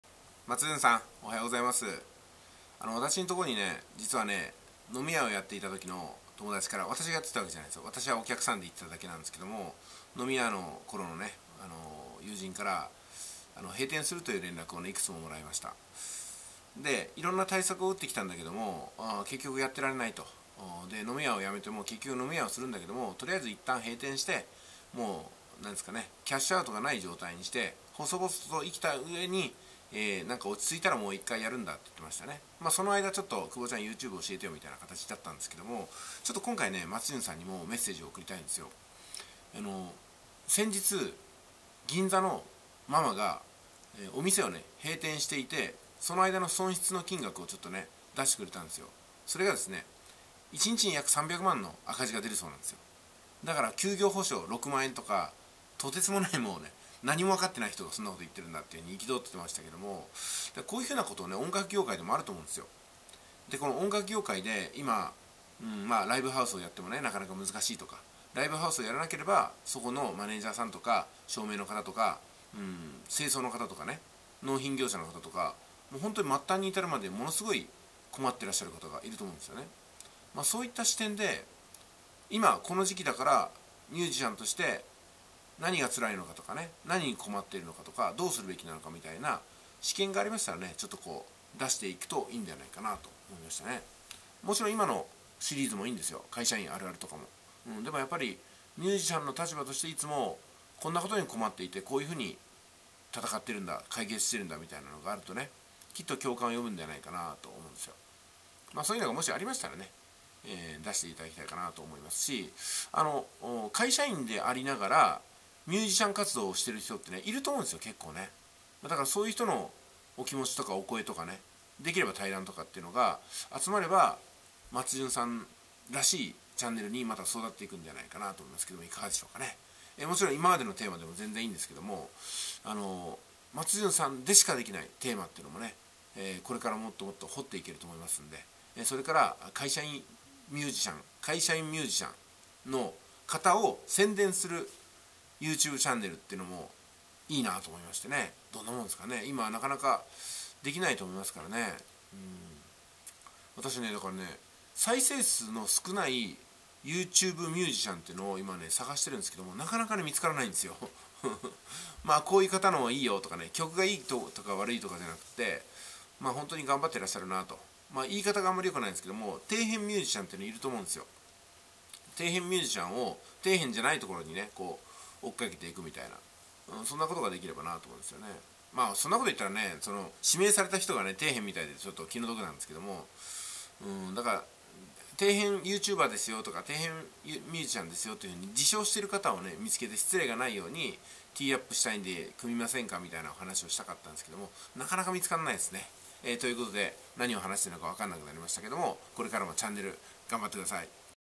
【松じゅん】第９９話：底辺ミュージシャンっているんですか？
底辺プロ17年ミュージシャンの収入源は？今月はやばかったが・・・。

「件名：速稼塾（あなたのお名前）」